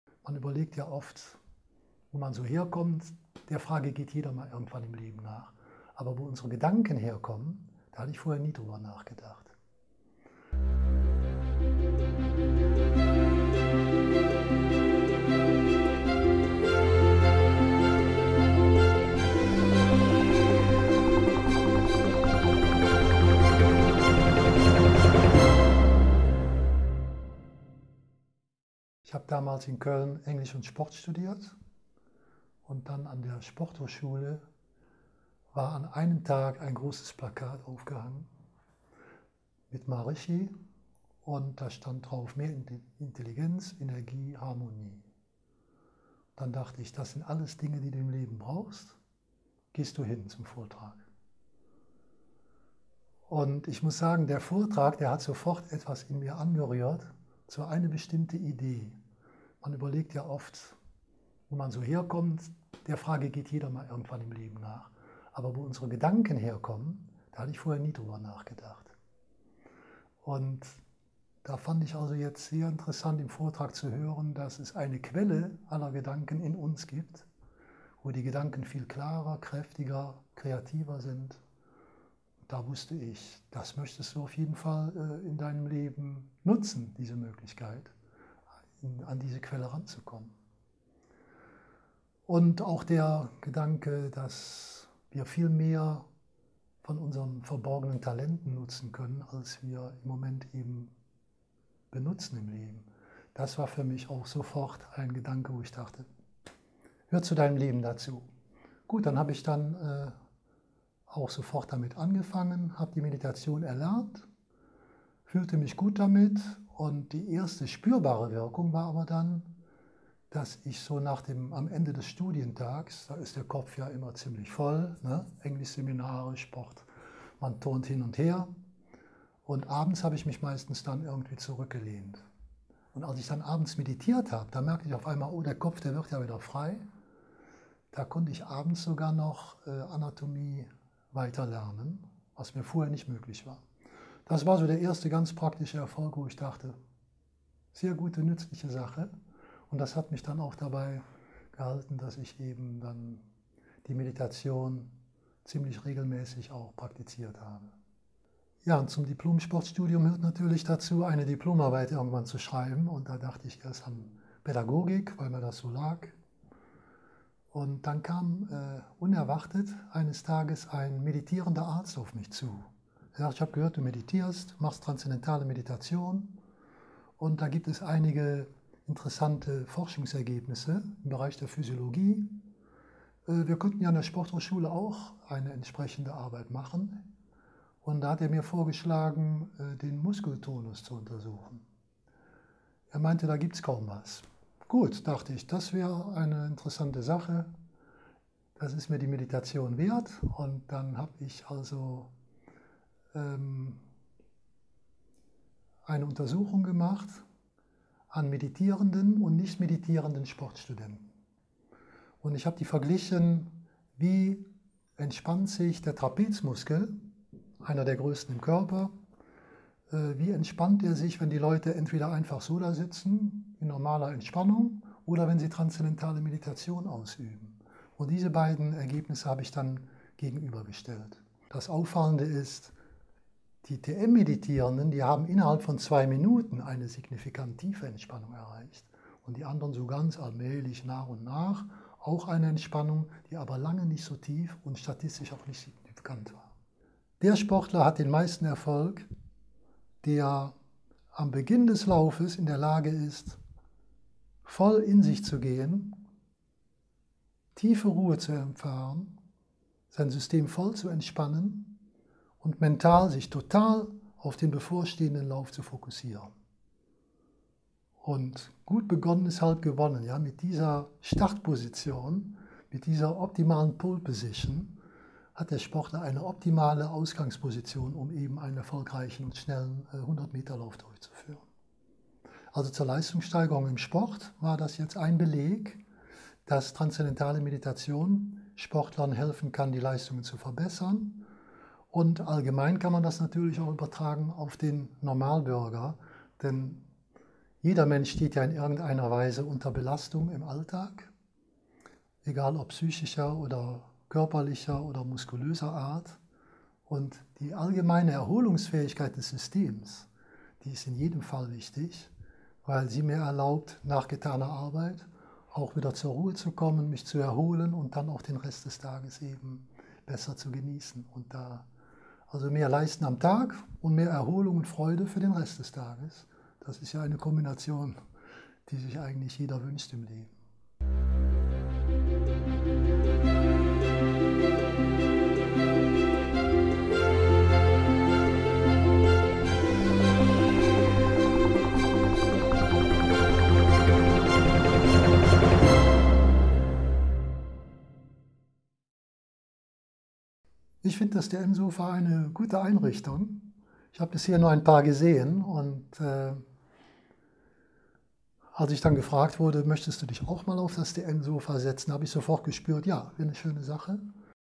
TM-Sofa: Sport und Meditation? Passt das zusammen?
Toni Kemmerling spricht über sein Sportstudium und seine Diplomarbeit zur Transzendentalen Meditation.

Toni Kemmerling hatte für seine Diplomarbeit an der Sporthochschule Köln die WIRKUNGEN DER TRANSZENDENTALEN MEDITATION AUF DEN MUSKELTONUS untersucht: Wie entspannt sich der Trapezmuskel,… entweder bei normaler Entspannung sitzend mit geschlossenen Augen oder wenn Transzendentale Meditation ausgeübt wird?

Das Auffallende ist, dass die TM-Meditierenden, innerhalb von zwei Minuten eine signifikant tiefe Entspannung erreichen, während Teilnehmer aus der Kontrollgruppe allmählich zwar auch zu einer Entspannung gelangen, diese aber nicht so tief ist und vor allem nicht die gleiche statistische Signifikanz zeigt.

„Zur Leistungssteigerung im Sport war das jetzt ein Beleg, dass Transzendentale Meditation Sportlern helfen kann die Leistung zu verbessern. Allgemein kann man das natürlich übertragen..., denn jeder Mensch steht ja in irgendeiner Weise unter Belastung im Alltag, egal ob psychischer, körperlicher oder muskulöser Art und die Erholungsfähigkeit des Systems, die ist in jedem Fall wichtig.“